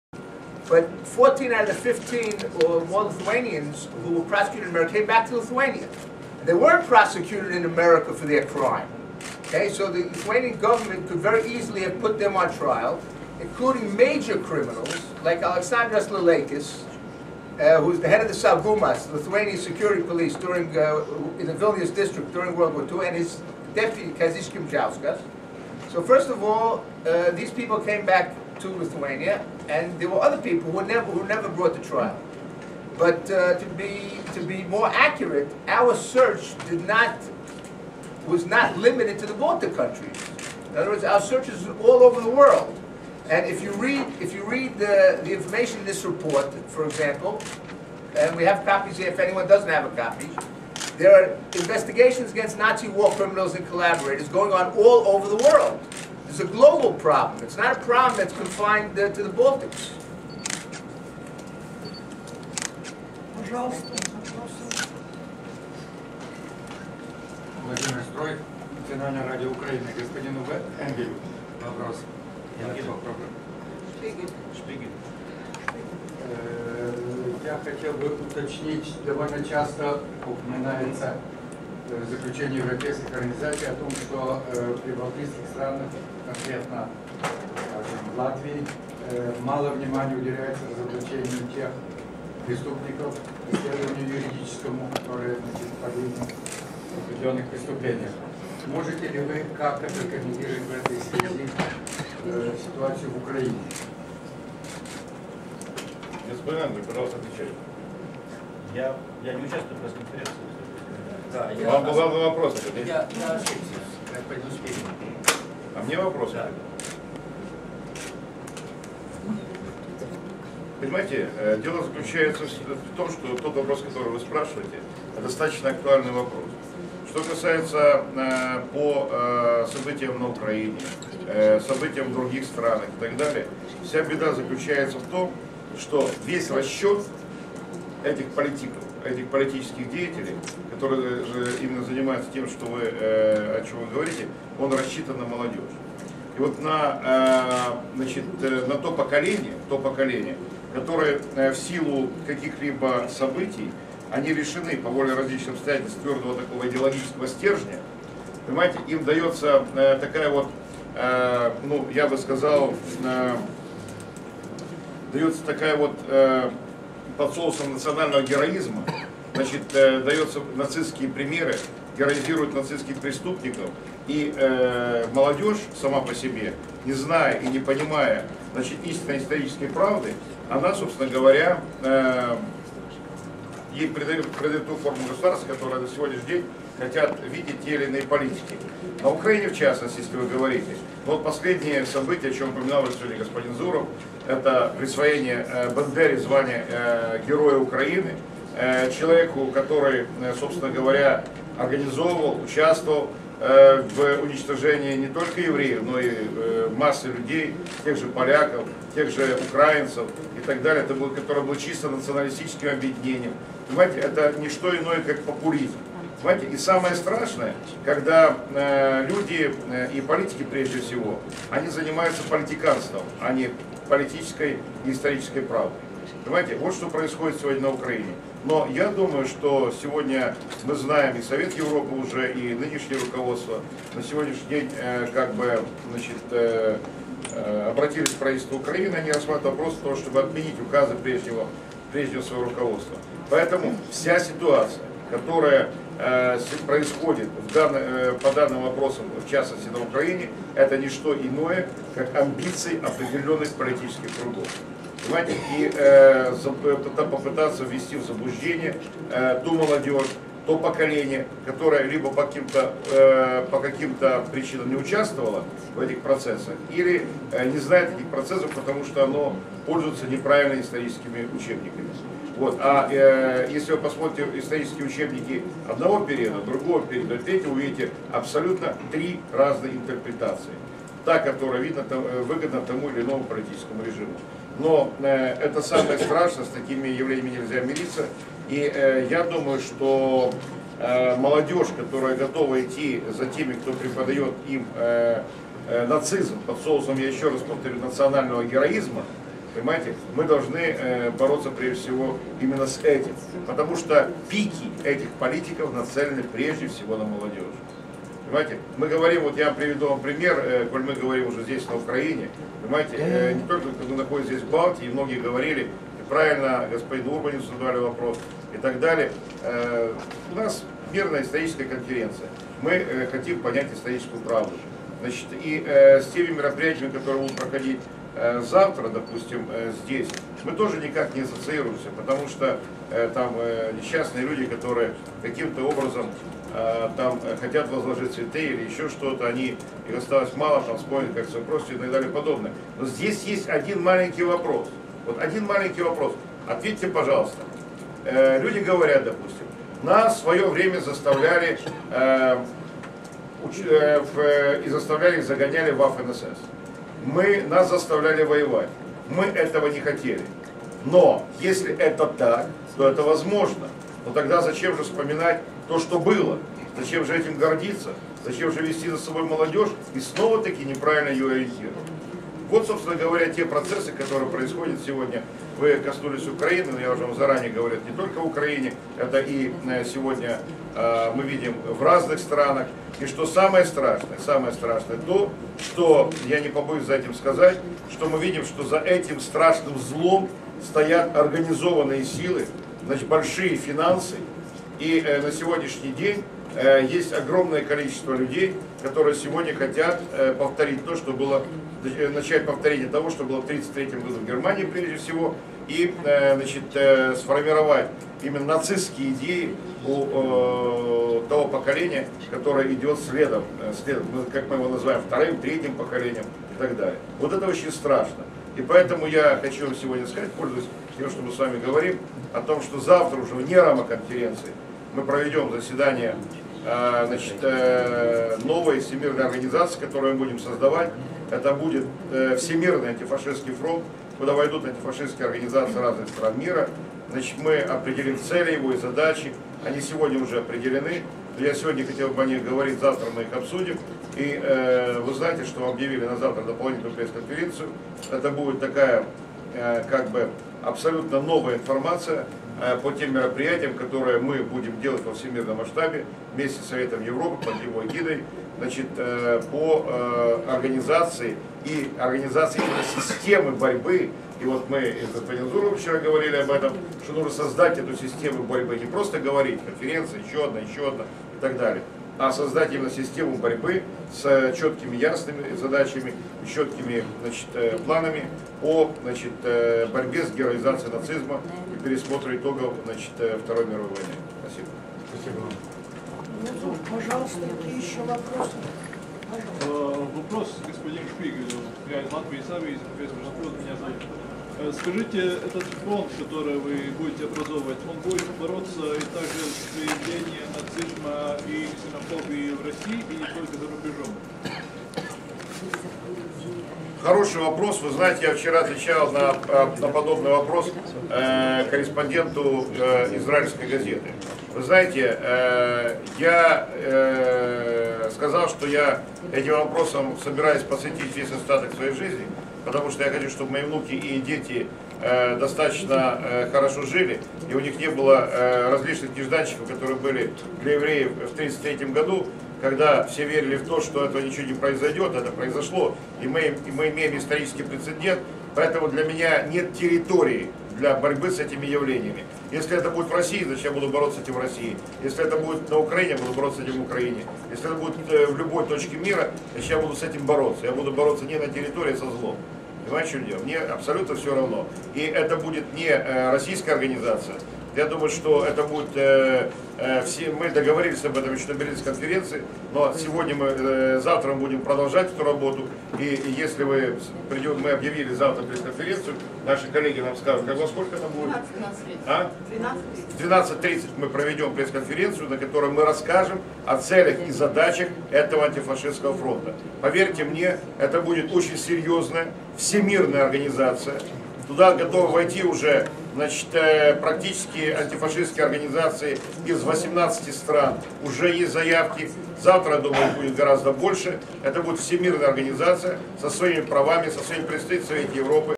WCRJ Part 2/5 - World Congress of Russian Jewry, Riga, Latvia 14-15 March 2010
At the press conference (mainly in russian): prof. Efraim Zuroff, congress president Boris Shpigel, prof. Abram Kletskin and Baltic Forum president Janis Urbanovics. The spirited party speach (in english) by prof. Dovid Katz. Camera, cooperation questions and love by Ditta Rietuma